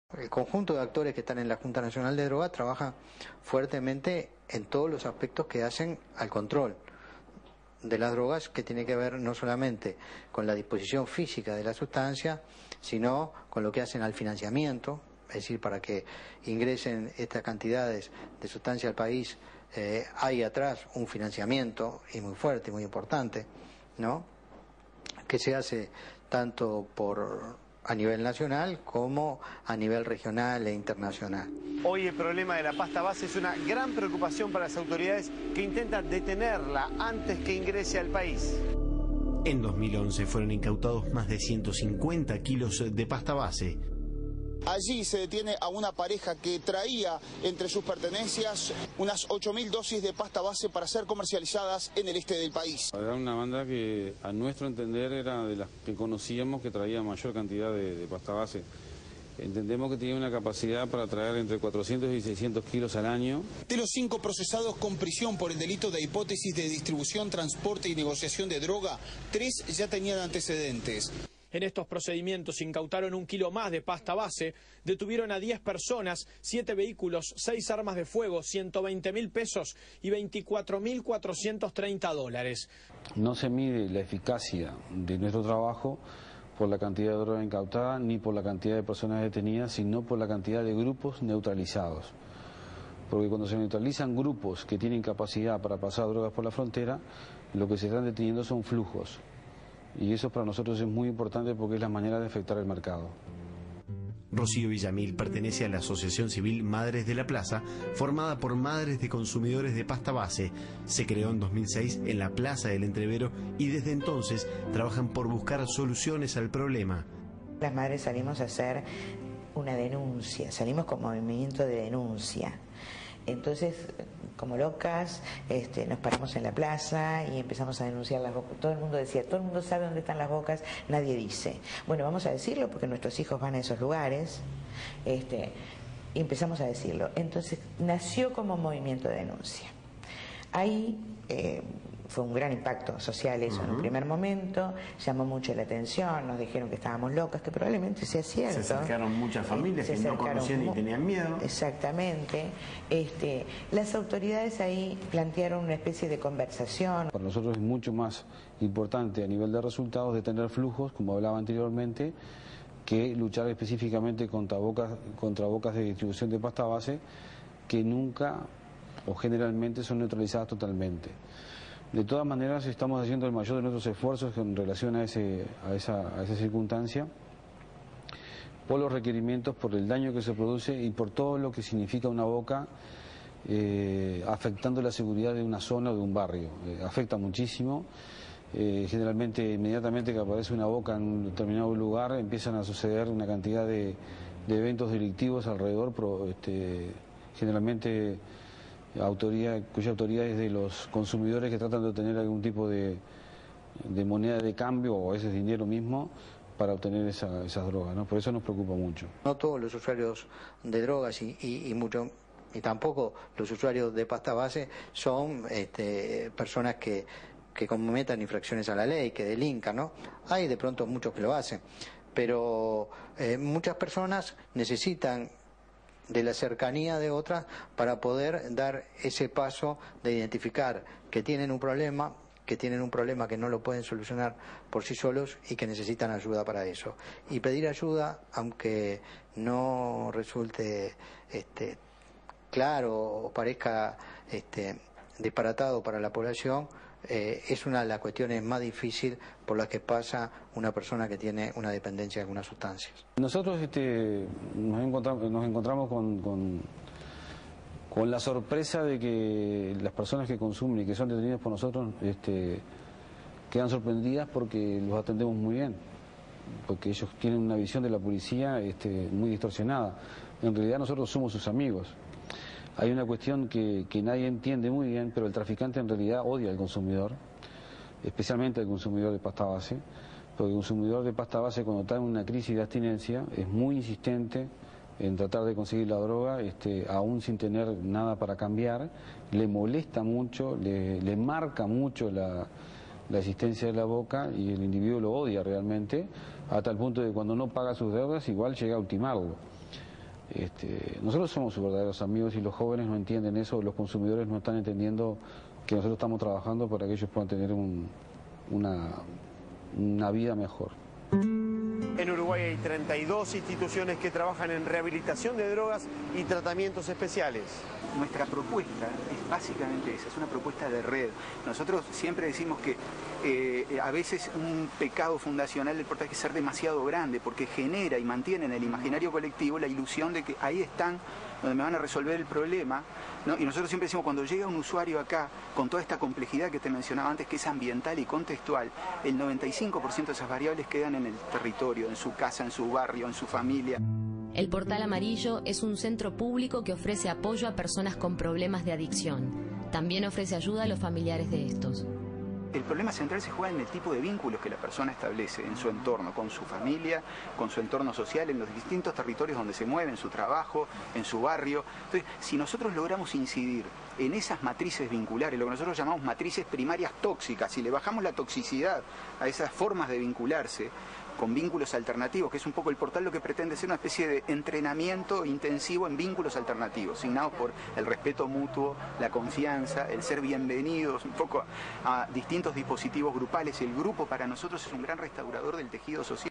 Los daños de la pasta base
Apareció en nuestro país-Uruguay-  en el año 2002, su consumo se concentra en las zonas con nivel socio económico más bajo. Una dosis cuesta $25 y el solo hecho de probar puede generar mucha adicción.

La pasta base es una gran preocupación para las autoridades que intentan frenar la droga antes que ingrese al país.